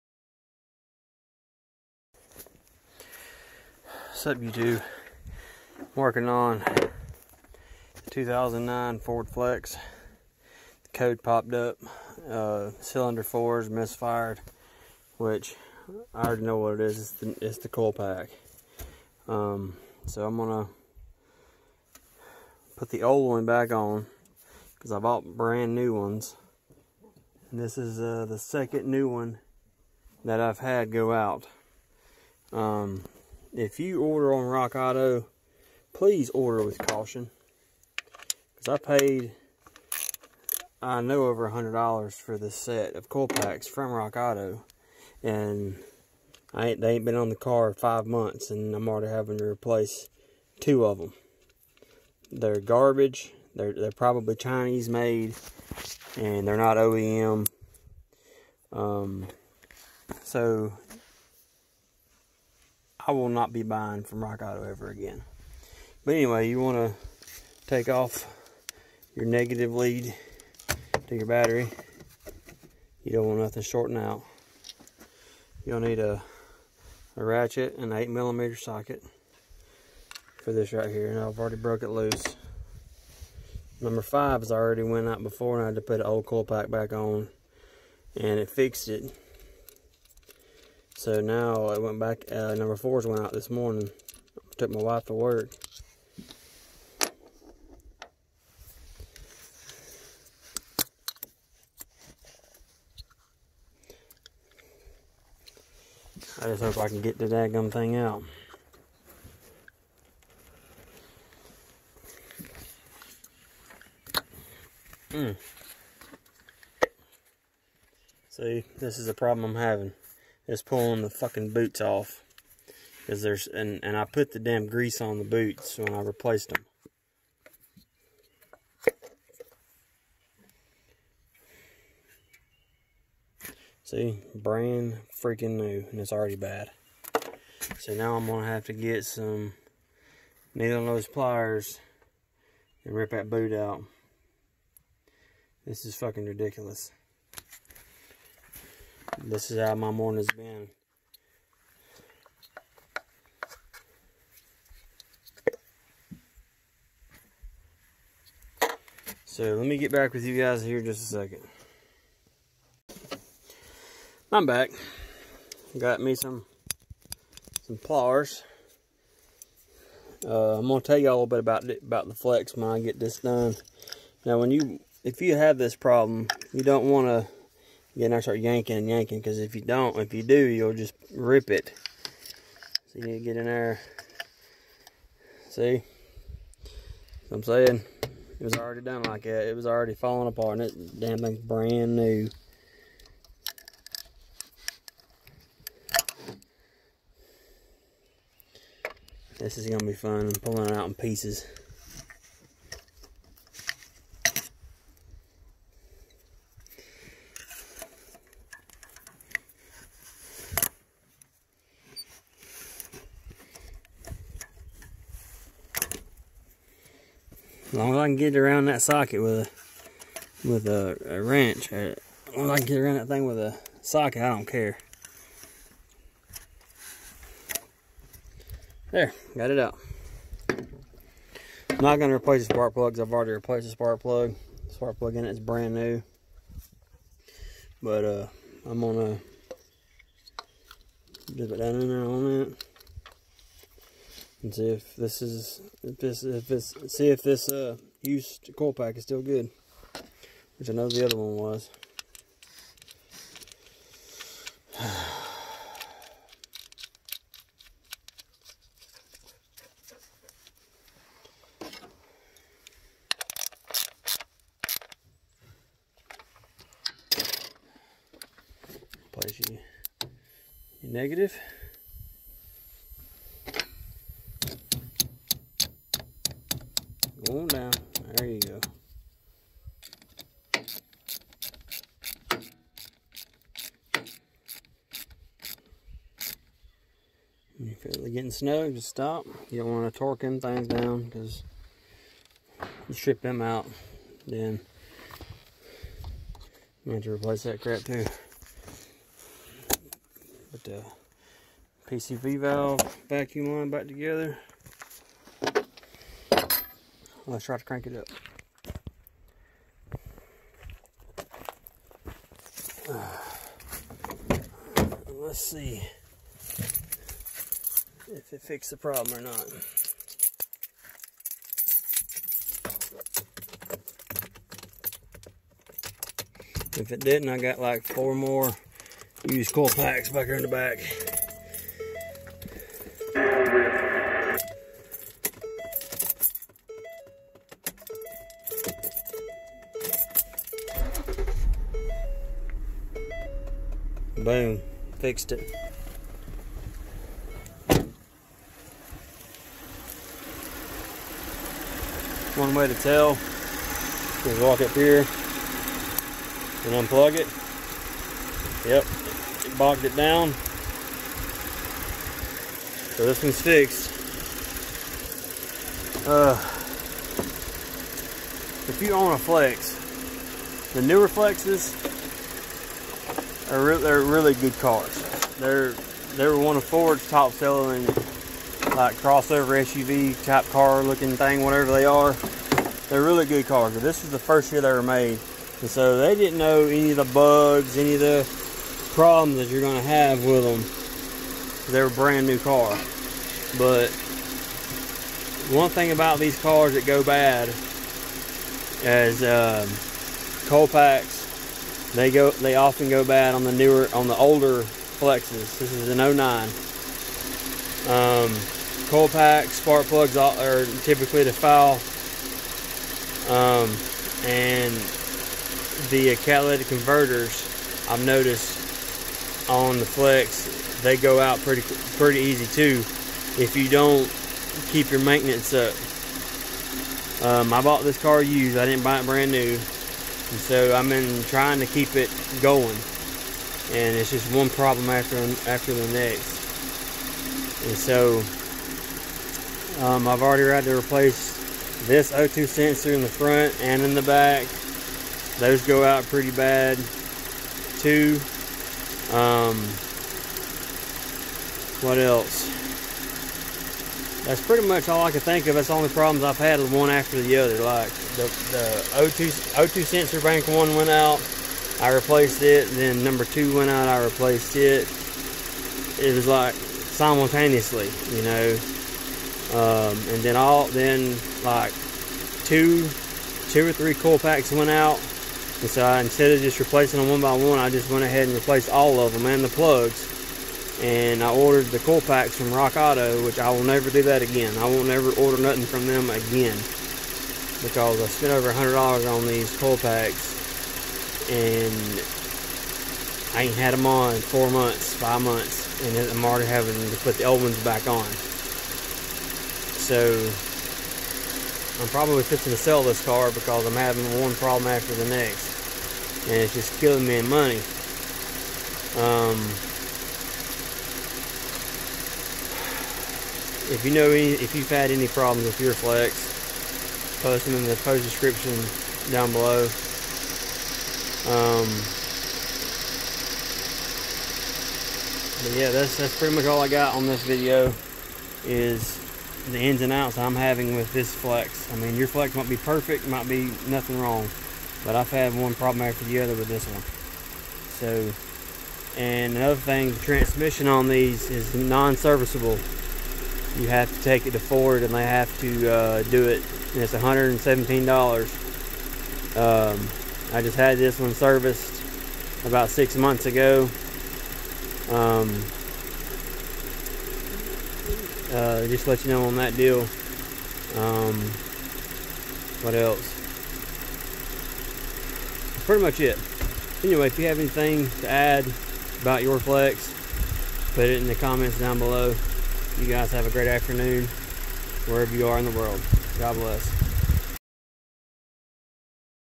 Ford Flex coil pack replacement.
Showing how to change number 4 coil pack being changed and what can happen.